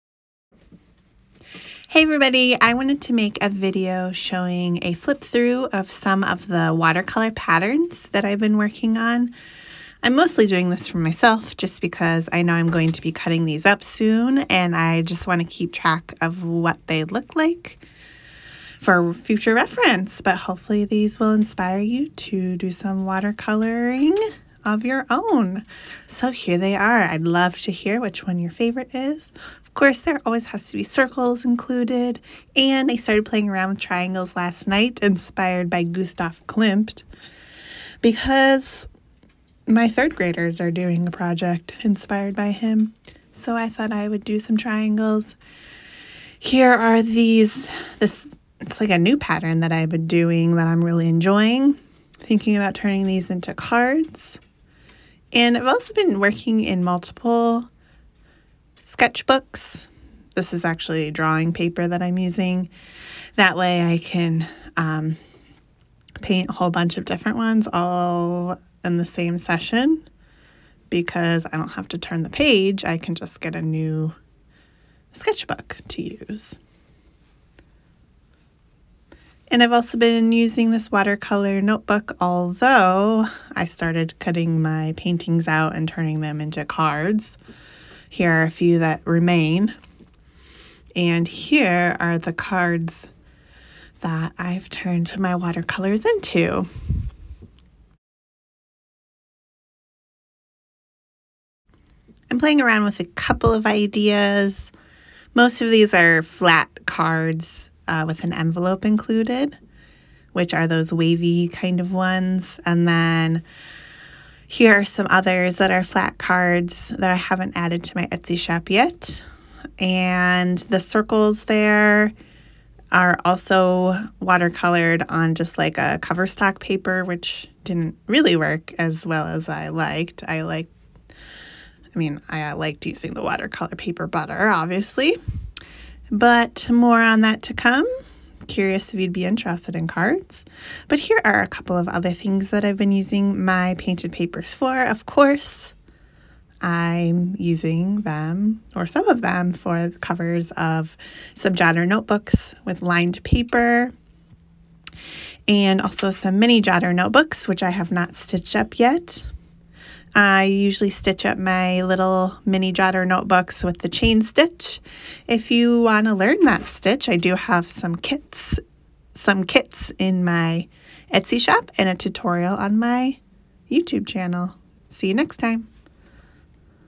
Watercolor Patterns Flip Through
Here's a low-key flip through of some of my recent watercolor patterns and what I'm thinking about using them for. What do you think? ❤️